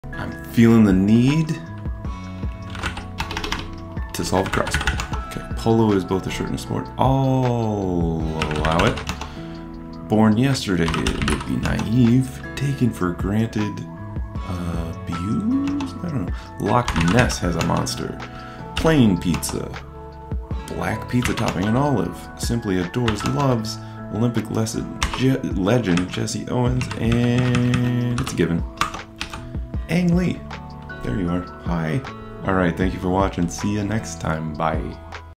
i cross words and you can too - Sun Dec 11 2022
@chadobear solves the Mini crossword